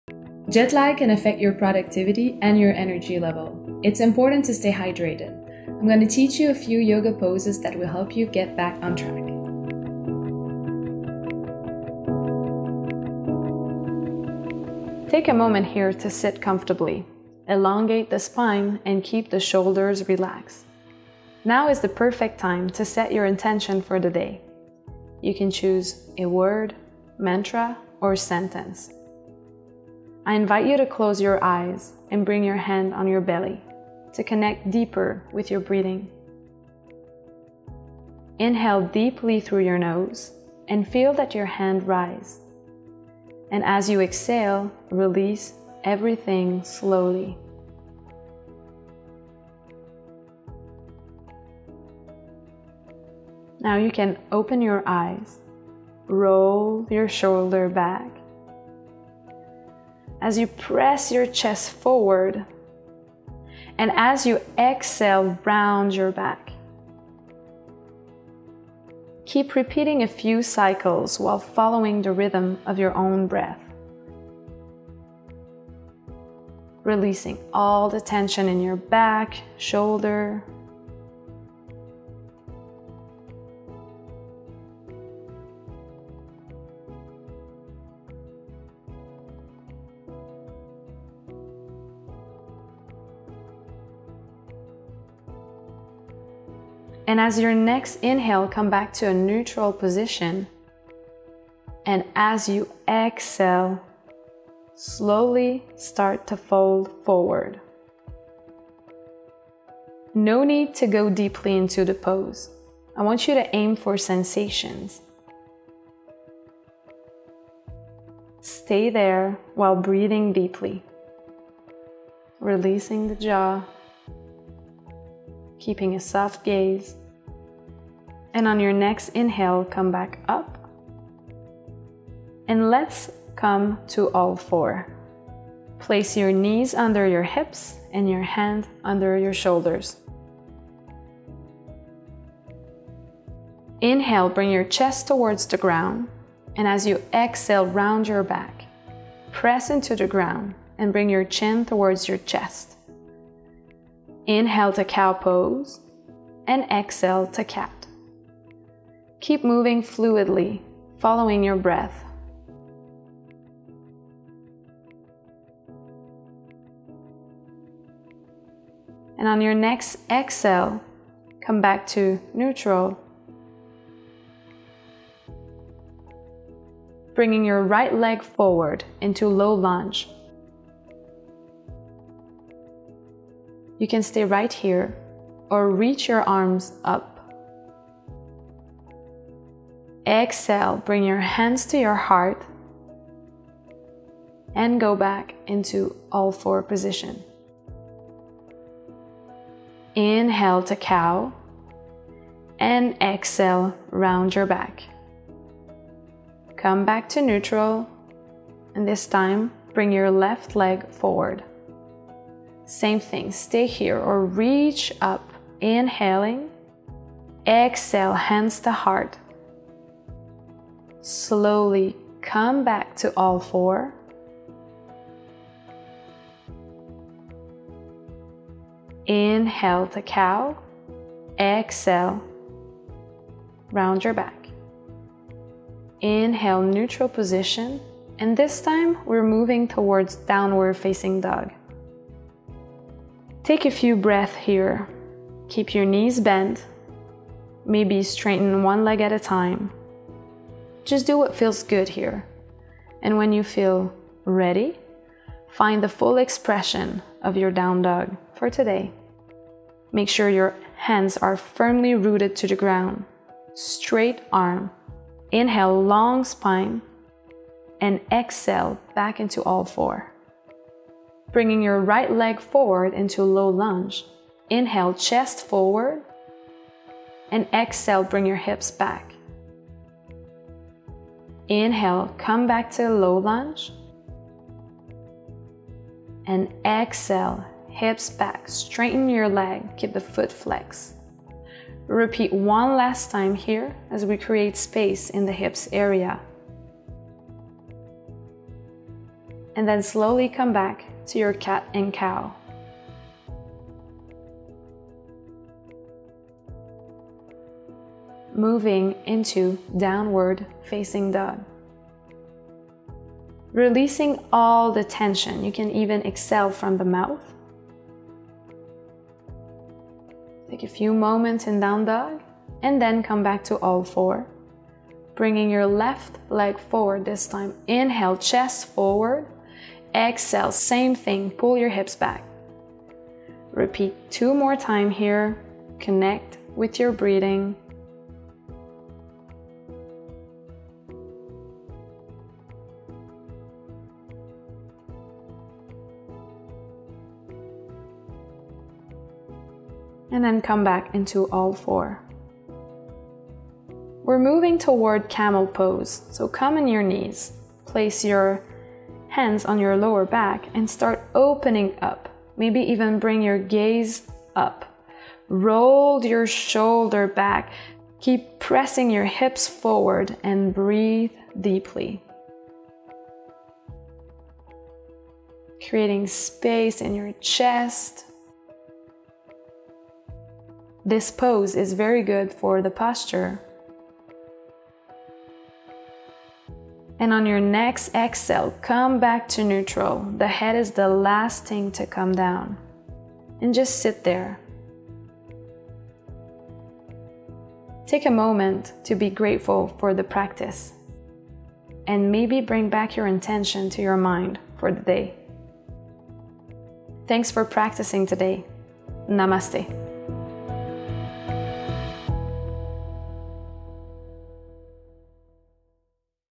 Nama-Stay by Germain Hôtels : Yoga to Cure Jet Lag
Frequent traveling often means jet lag. With meetings to go to and places to see, a quick yoga session with our collaborator Virginie Duval can help you cure jet lag and be energized for the day. Follow the steps!